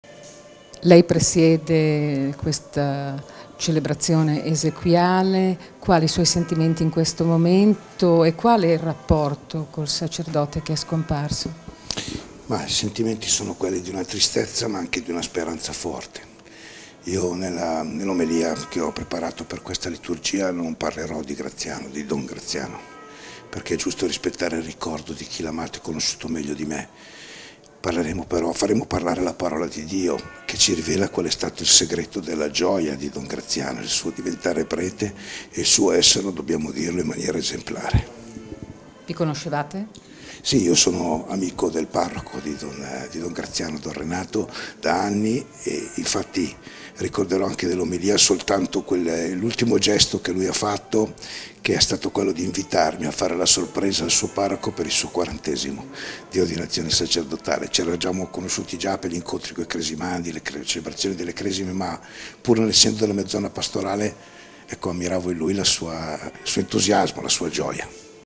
15 luglio 2021 Alla Bovisa i funerali don Graziano Gianola - intervista a mons. Raimondi
Questa mattina, nella chiesa parrocchiale di Santa Maria del Buon Consiglio a Milano, monsignor Luca Raimondi, Vescovo ausiliare, ha presieduto i funerali di don Graziano Gianola, il 48enne sacerdote ambrosiano morto nel pomeriggio di lunedì 12 luglio all’ospedale Santa Chiara di Trento, a seguito delle gravi ferite riportate in una caduta in montagna, mentre si trovava in vacanza a Brentonico con una sessantina di ragazzi del suo oratorio e una suora.

Prete dal 2004, dopo le prime esperienze nelle parrocchie di Sesto San Giovanni, Buccinasco e Assago, dal 2013 don Graziano era Vicario parrocchiale in Santa Maria del Buon Consiglio (dal 2014 unitasi alla parrocchia dei Santi Giovanni e Paolo nella Cp Gesù Buon Pastore). Era anche docente di religione cattolica nella scuola secondaria di primo grado, formatore nell’Istituto secolare delle Apostole del Sacro Cuore e autore di volumi pubblicati da Ancora.

Don Gianola era nato a Premana (Lc), dove domani mattina, alle 10, riceverà l’estremo saluto in una celebrazione presieduta dall’Arcivescovo, monsignor Mario Delpini.